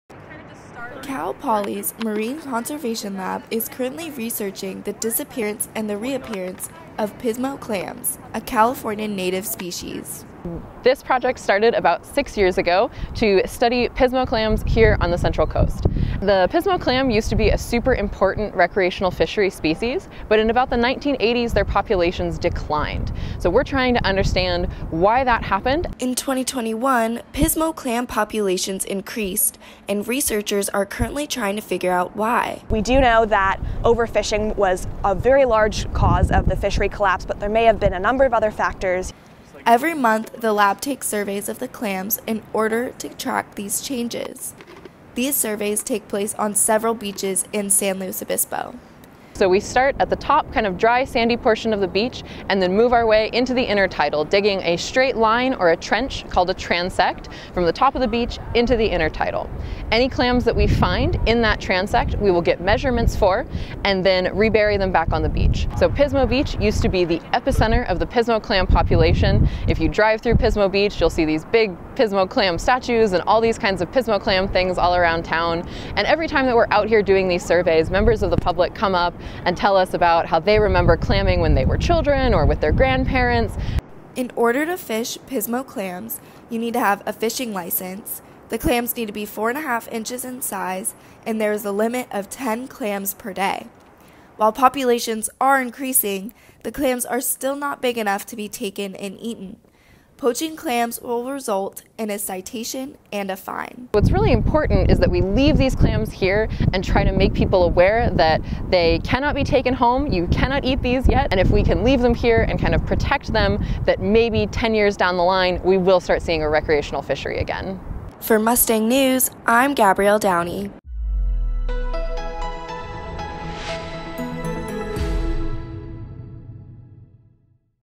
How The Pismo Clam is Bouncing Back
A staple at Pismo Beach, the Pismo Clam began disappearing in the 1980s. But in 2021, scientists began to notice that the clams are back --- but they don't know why. Mustang News Reporter Gabrielle Downey has more on the marine phenomenon.